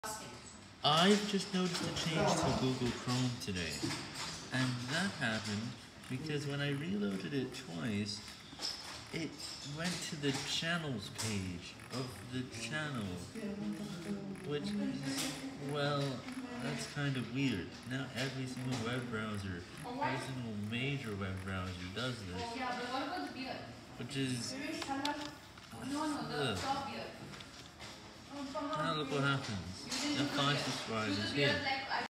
Something changed.
I saw a change to the Google Chrome browser with channel pages when you’re on a channel section. After two reloads, it redirects you to “channels“.